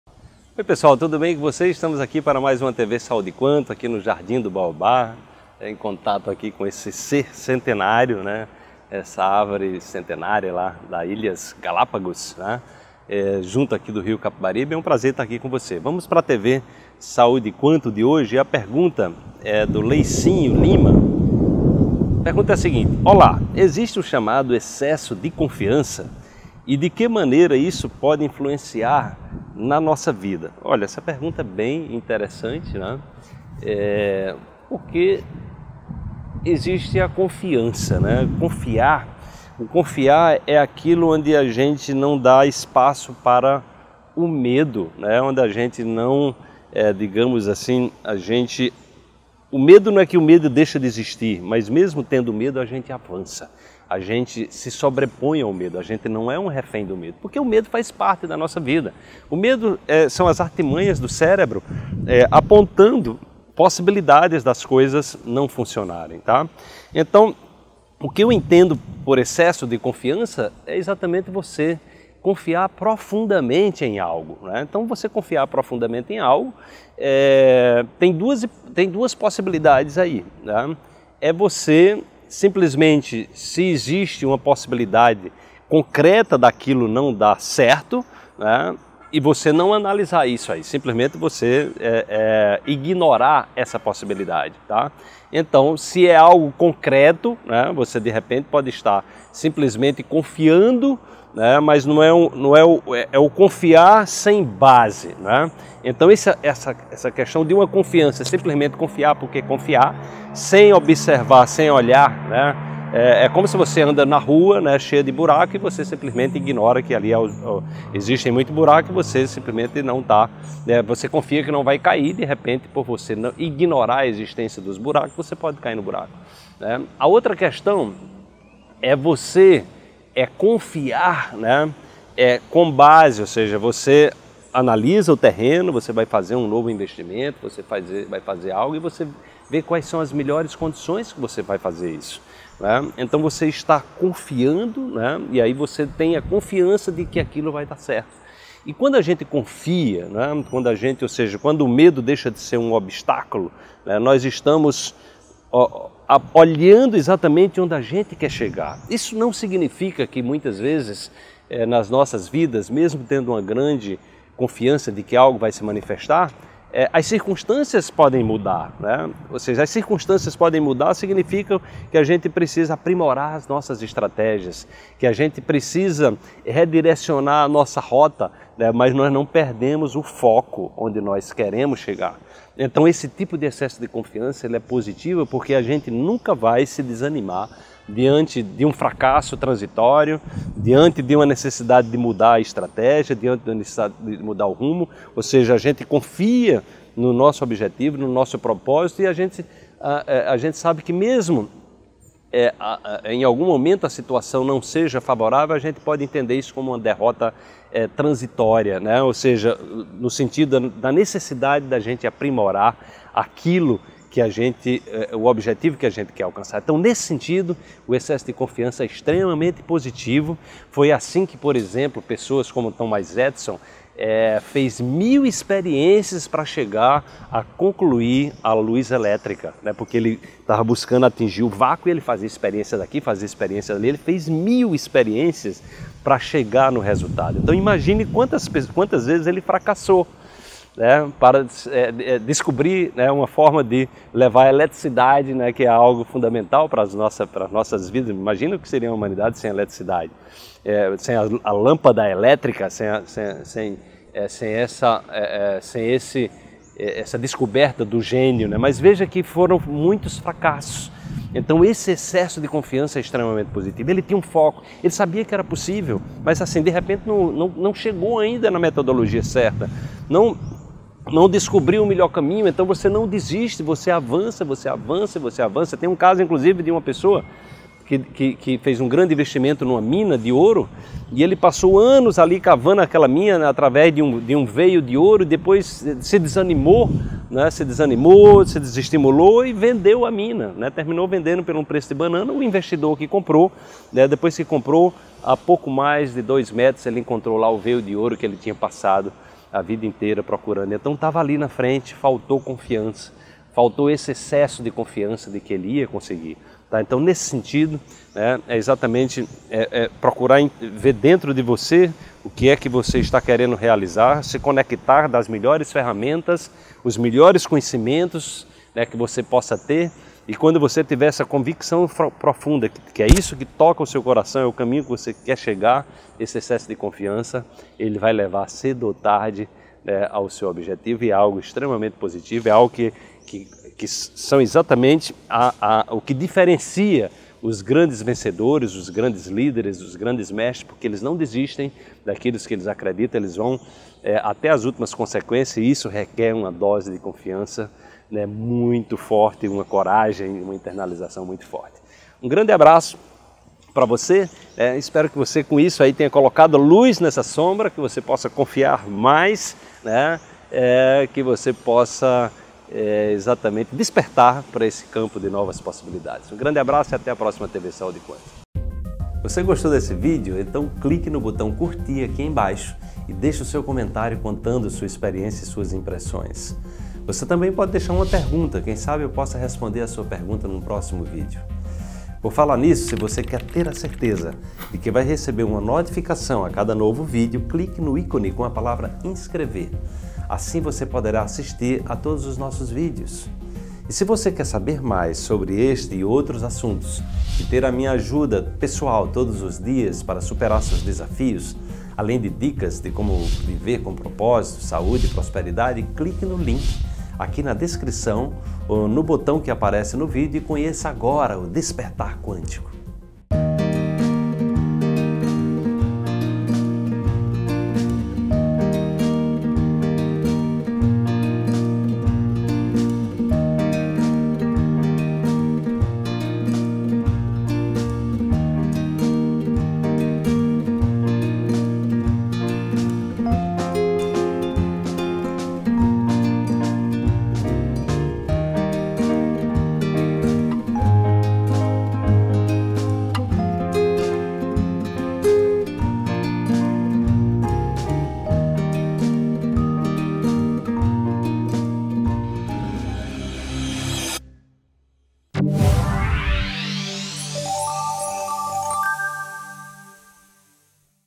TV Saúde Quantum 090: É bom ou Ruim ter Excesso de Confiança? | Wallace Liimaa
✭✭✭✭✭ É bom ou Ruim ter Excesso de Confiança. Nesse vídeo abordo a importância da confiança profunda em algo concreto para alcançar um objetivo. Assista o vídeo e comprove!